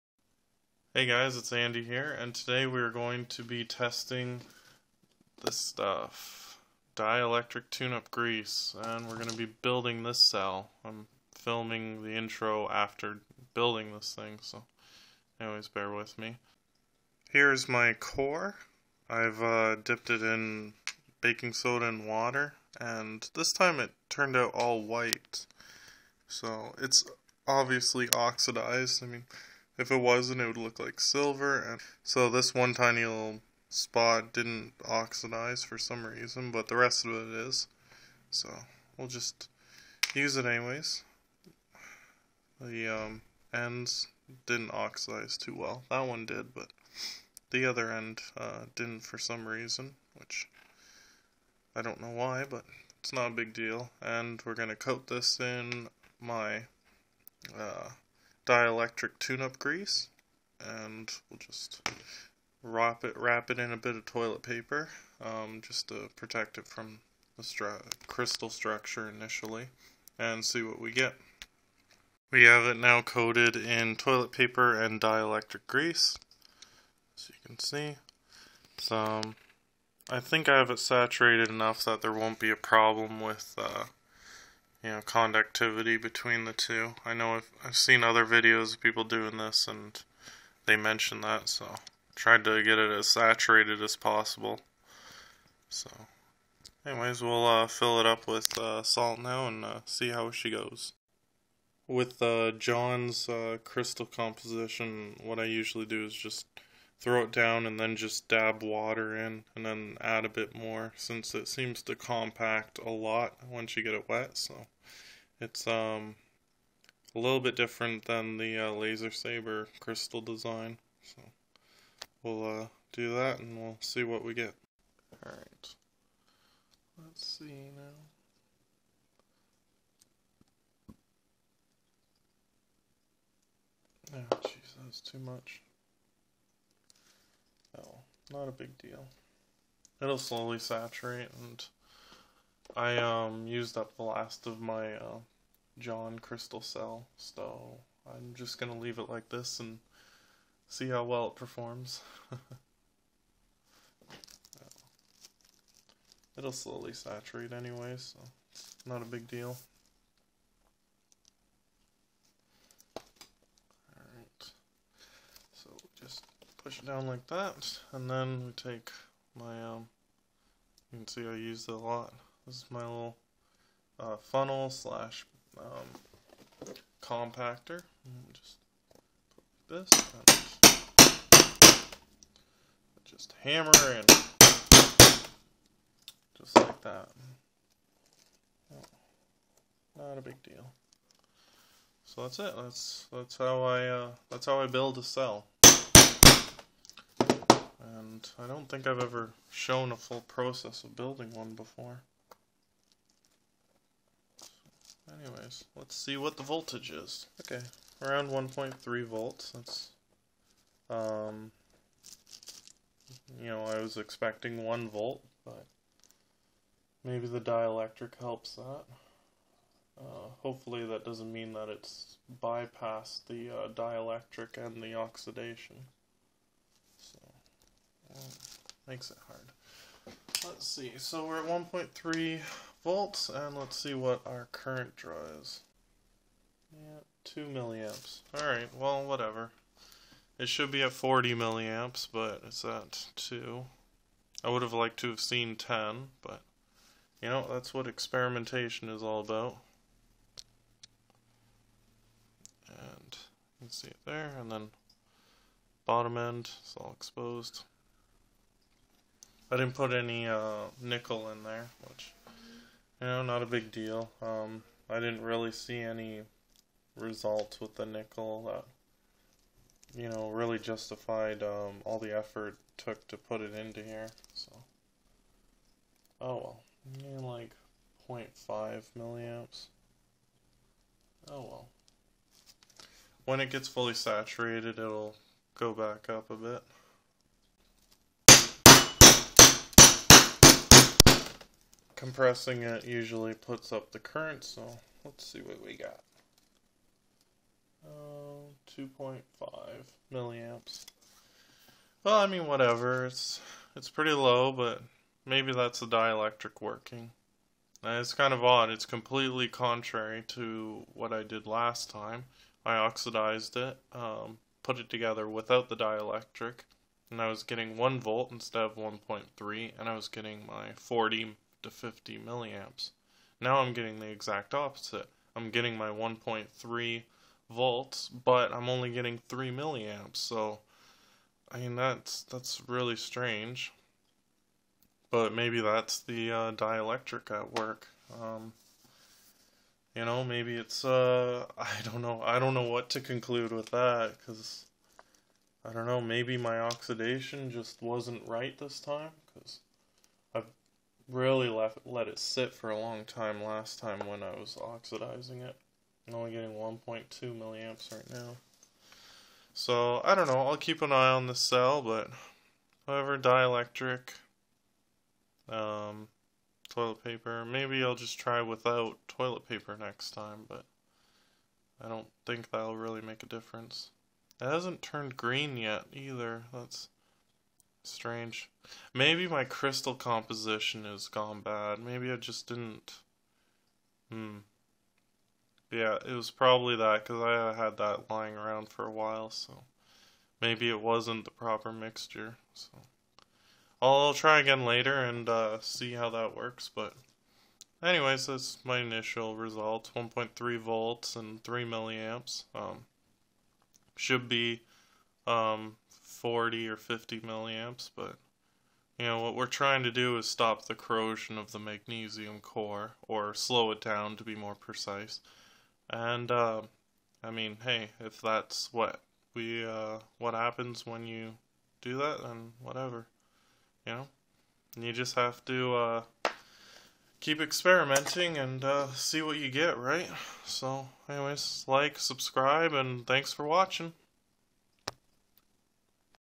12. Crystal Cells build process & testing dielectric grease
In this video I show my build process of creating a crystal cell as well as testing the effects of coating the magnesium core in dielectric grease. The hope is that this will slow the corrosion and we will tear it apart in 7 days and find out.